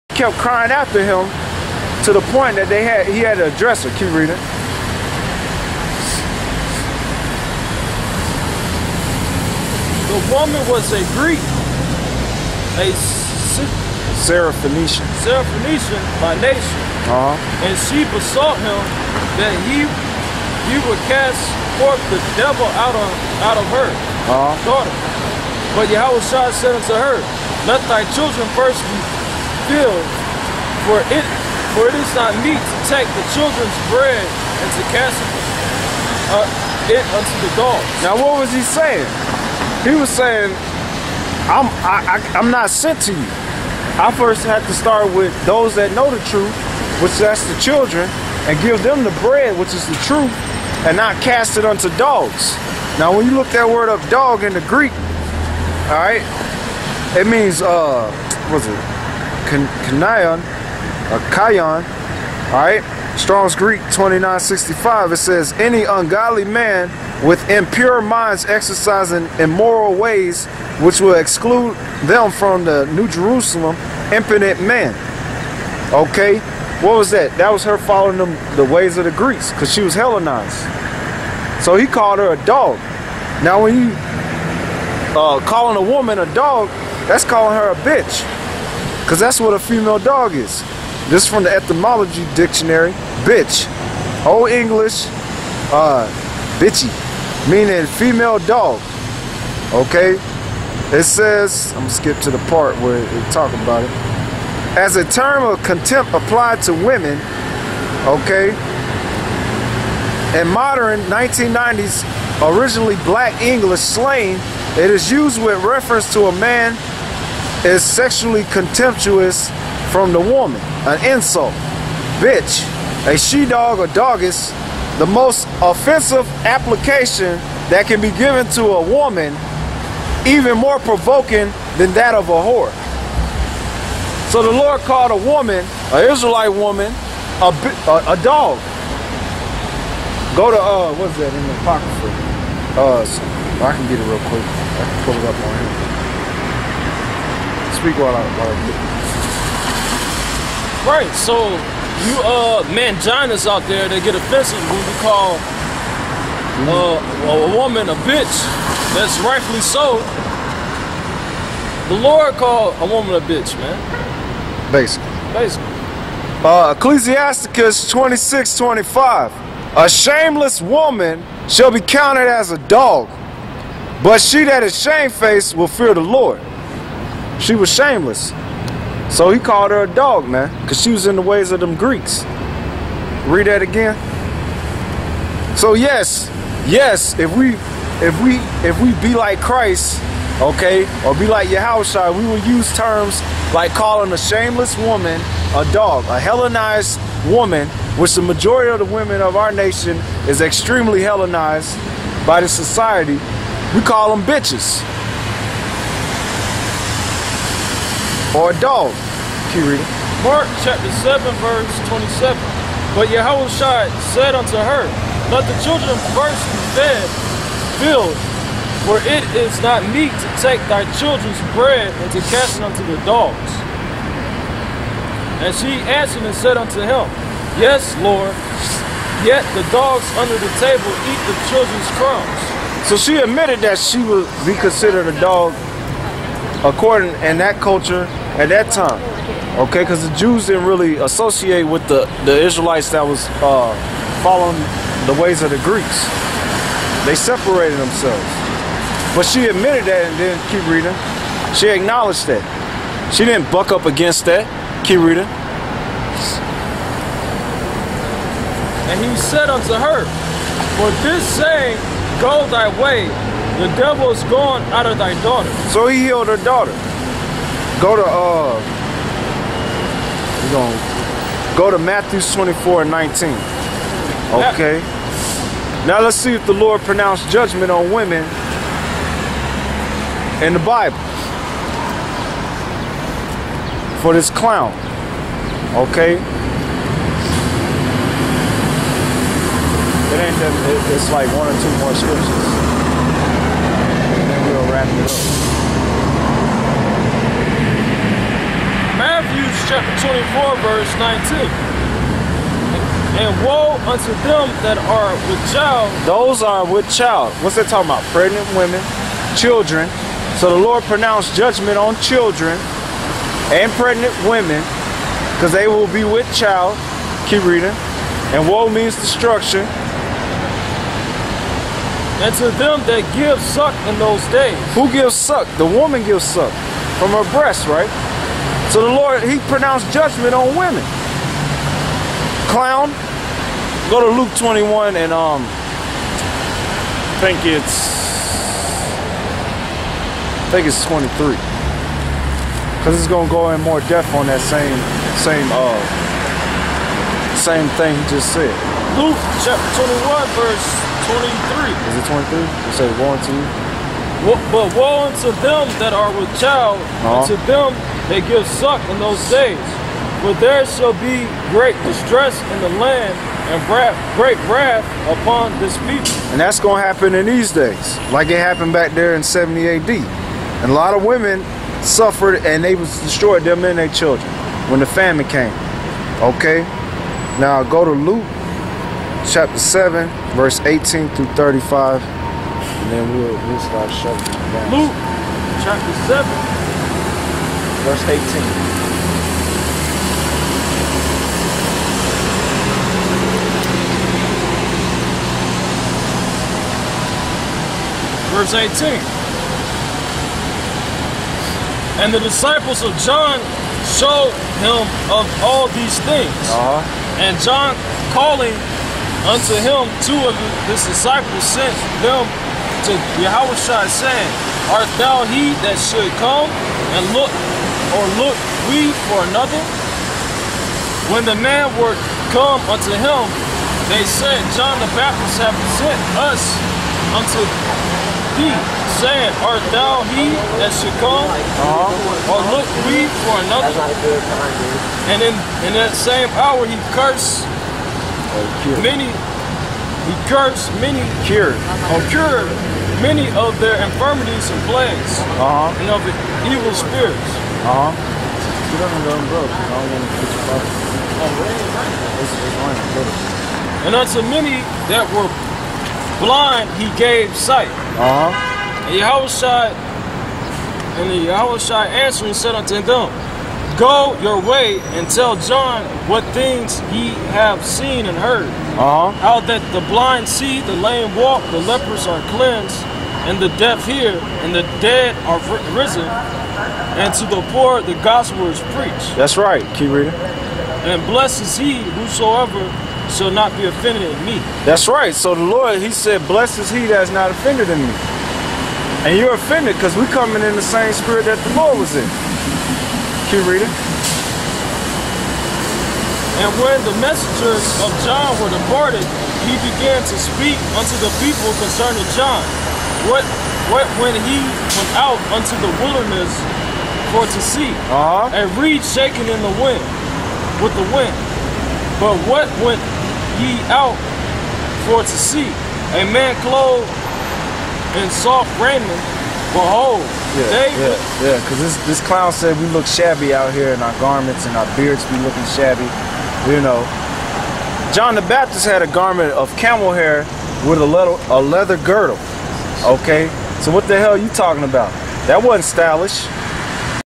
PT9 The Lord Called An Woman An Dog, Which Is A Bitch! Cut On The 14th Street Israelites!
Mark 7:26-27, Ecclesiasticus 26:25, Mark 7:27-30, Matthew 24:19, Luke 21:23, Luke 7:18-25